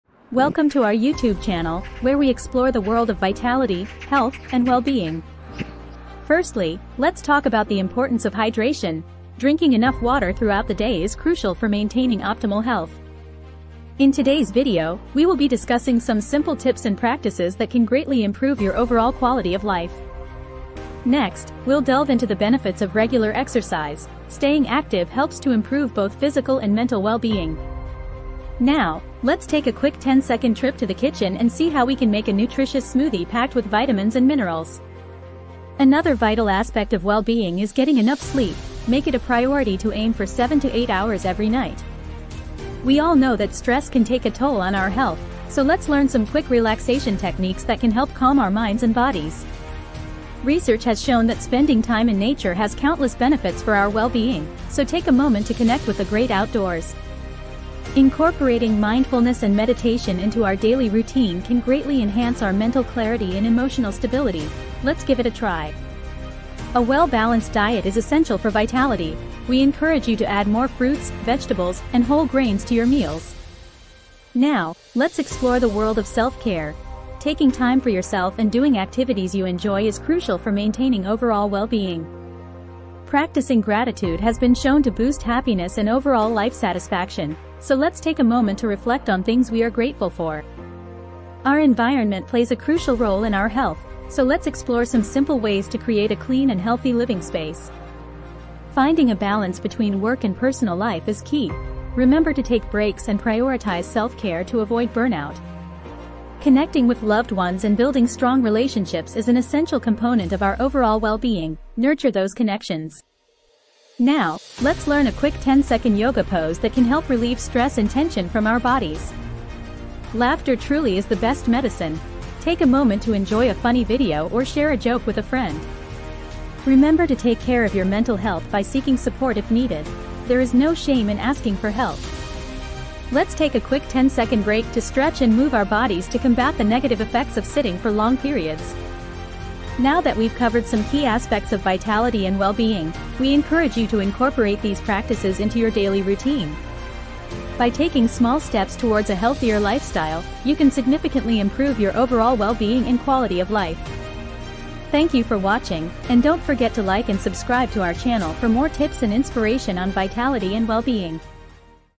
Optimize Your Health: 20 Easy Tips for Vitality and Well-Being
Welcome to our empowering YouTube channel, where we unlock secrets to a vibrant life! Discover 20 simple tips for vitality, health, and well-being. Learn the importance of hydration, exercise, and nutritious smoothies. Prioritize 7-8 hours of restful sleep and practice quick relaxation techniques to combat stress. Embrace nature, mindfulness, and meditation for mental clarity. Enjoy a well-balanced diet with grains and indulge in self-care. Boost happiness through gratitude and create a healthy living space. Balance work and personal life, nurture relationships, and laugh for joy. Seek support for mental health and stay active with 10-second stretches. Elevate your life with these transformative practices.